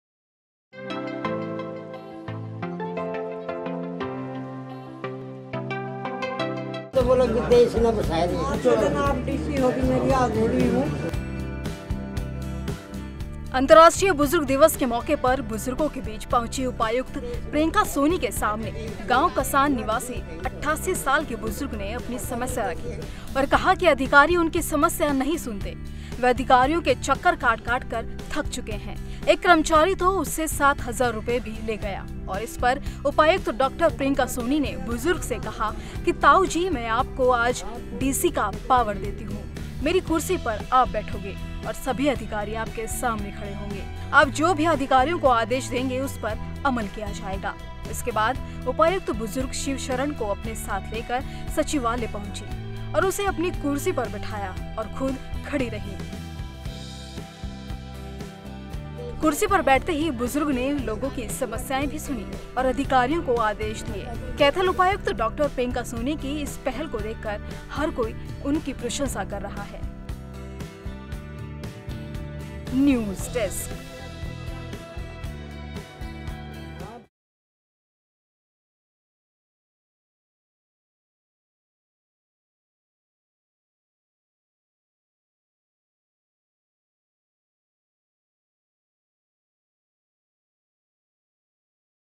नहीं हो रही थी 88 साल के बुजुर्ग की सुनवाई, महिला अधिकारी ने बना दिया एक दिन का DC
अंतर्राष्ट्रीय बुजुर्ग दिवस के मौके पर बुजुर्गों के बीच पहुंचीं उपायुक्त प्रियंका सोनी के सामने गांव कसान निवासी 88 साल के बुजुर्ग ने अपनी समस्या रखी और कहा कि अधिकारी उनकी समस्या नहीं सुनते. वह अधिकारियों के चक्कर काट-काटकर थक चुका है. एक कर्मचारी तो उससे 7 हजार रुपए भी ले गया. इस पर उपायुक्त डॉ. प्रियंका सोनी ने बुजुर्ग से कहा कि ताऊ जी आज मैं आपको डीसी की पावर देती हूं. मेरी कुर्सी पर आप बैठोगे और सभी अधिकारी आपके सामने खड़े होंगे. आप जो भी अधिकारियों को आदेश देंगे, उस पर अमल किया जाएगा.